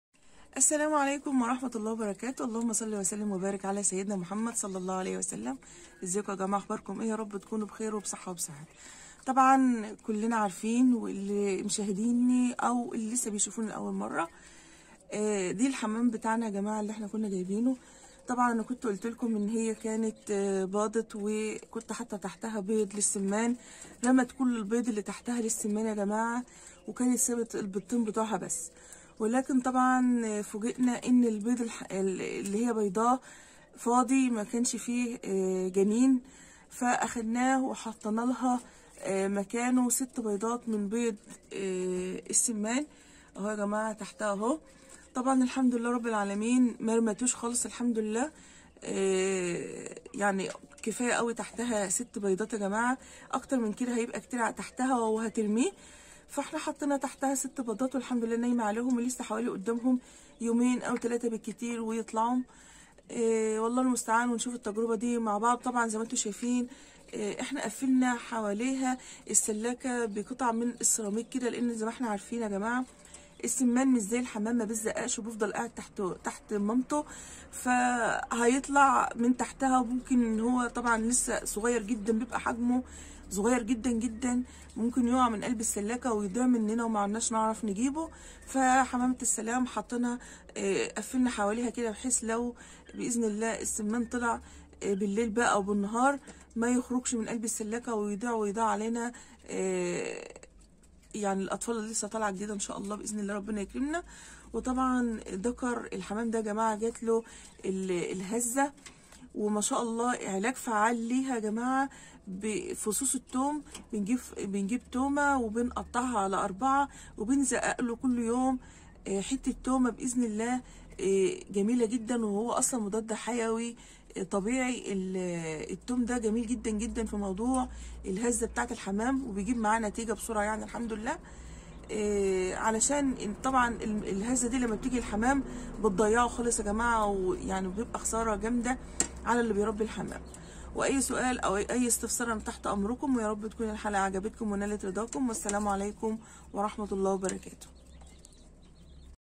تعالو شوفو بيض السمان تحت الحمامة وقرب يطلع ادعولنا
لا تنسوا الاشترك بالقناة احبائي ليصلكم مني كل جديد
ما عليك سوى الانتساب إلى هذه القناة للاستفادة من المزايا: